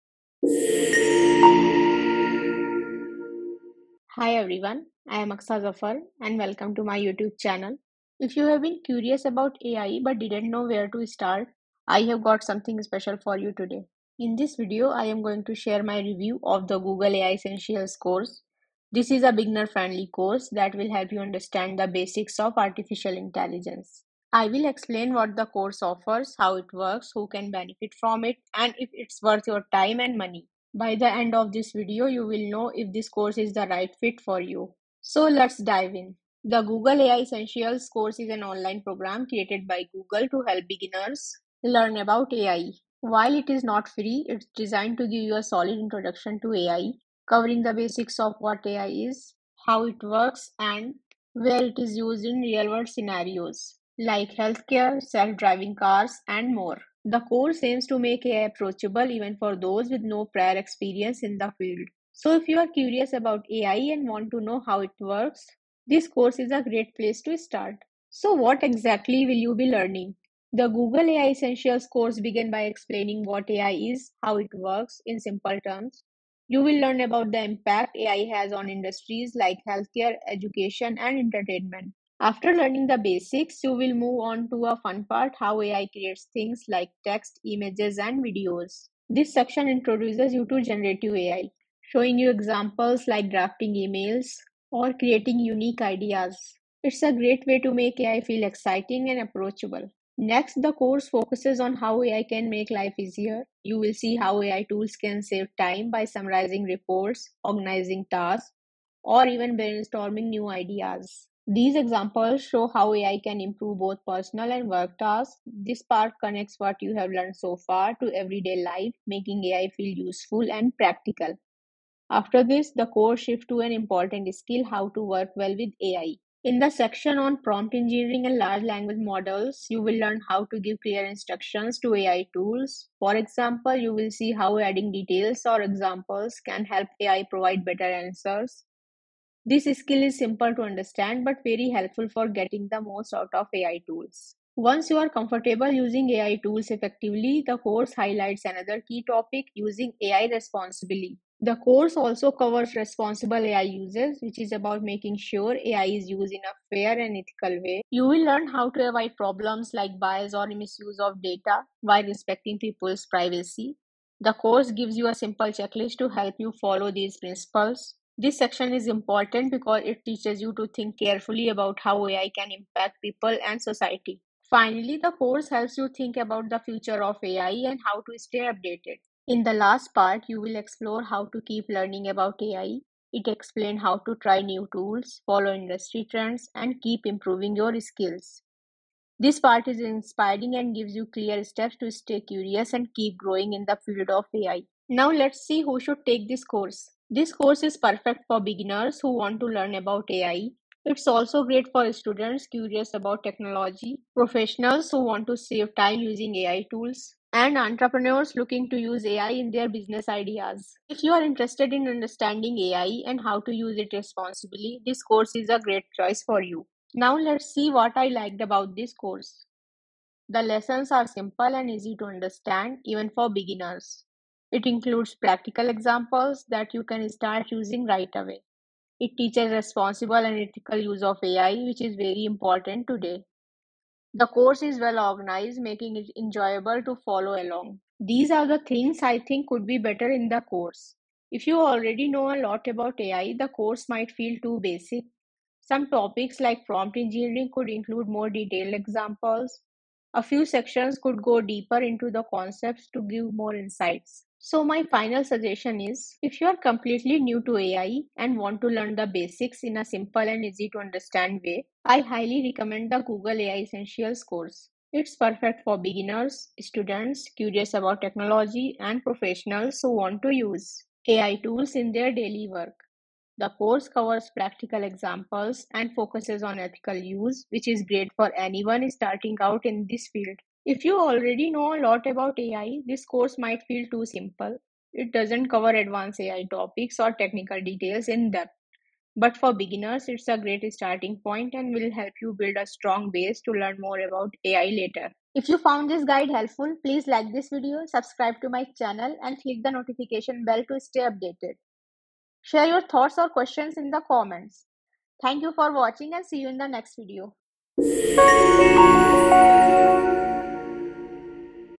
Google AI Essentials Course Review: Is It Worth Your Time & Money?🔍(My Honest Experience)
Thinking about learning AI but unsure where to start? In this detailed video, I’m sharing my complete Google AI Essentials Course Review, breaking it down in a simple, easy-to-follow way.

Wondering, "What is Google AI Essentials Course?" or "Is Google AI Essentials worth it?" I’ll answer these questions and more based on my personal experience.

📚 What you'll learn in this video:
-What the Google AI Essentials Course is all about.
-The topics covered, like AI basics, daily productivity hacks, and responsible AI use.
-My honest review: The good, the not-so-good, and whether it’s worth your time.
-How this course can help beginners and professionals in their careers.

Whether you're curious about AI or looking for tools to boost your productivity, this video will help you decide if this course fits your goals. Watch until the end for insights that could save you time and money!

🎓 Ready to enroll?

Timestamps

0:00 Introduction
0:37 Course Intro
1:08 What You Will Learn?
3:15 Who Should Take This Course?
3:37 What I Liked About the Course
3:58 What Could Be Better
4:14 My Final Suggestion